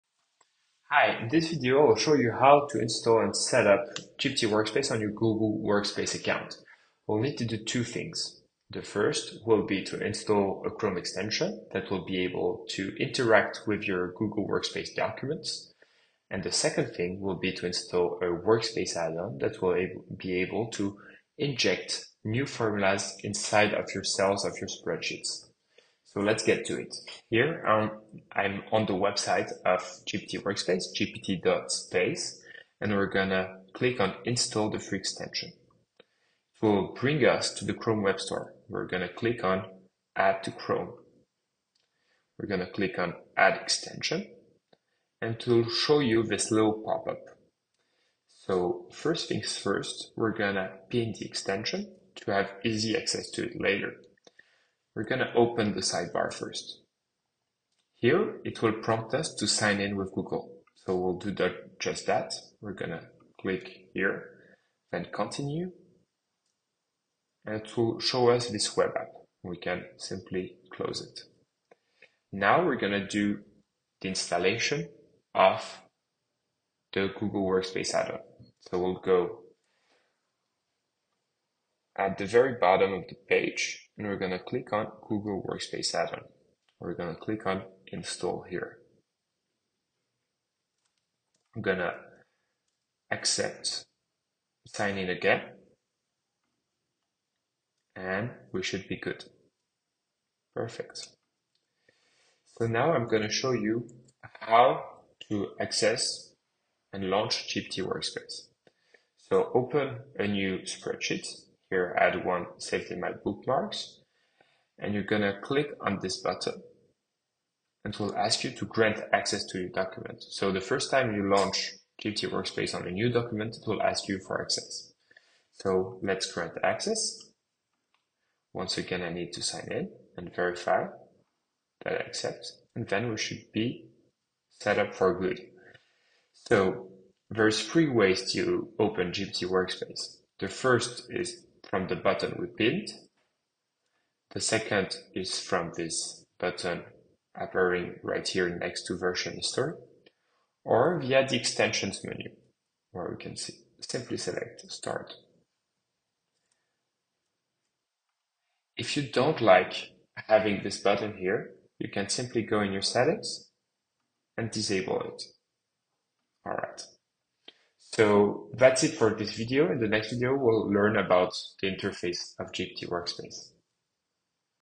1.1 How to install and setup GPT Workspace | Guide for Google Docs, Sheets, Slides, and Gmail
How to Install and Set Up GPT Workspace on Google Workspace | GPT for Sheets, Docs, Slides, and Gmail

In this video, we'll walk you through the easy steps to install and set up GPT Workspace on your Google Workspace. Boost your productivity with the power of AI across Google Sheets, Google Docs, Google Slides, and Gmail. Discover how you can use GPT-powered formulas in Sheets, auto-generate text in Docs, create stunning slideshows, and manage your emails smarter with GPT in Gmail.

✅ Get Started with GPT Workspace:

Chrome Extension: GPT Workspace Chrome Extension
Google Workspace Add-on: GPT for Docs, Sheets, Slides
GPT Workspace Features:

AI-powered formulas in Google Sheets
Generate and improve text in Google Docs
AI-driven slide creation in Google Slides
Smarter email replies and insights in Gmail
Whether you're managing data, creating presentations, or handling emails, GPT Workspace makes your workflow seamless and more efficient!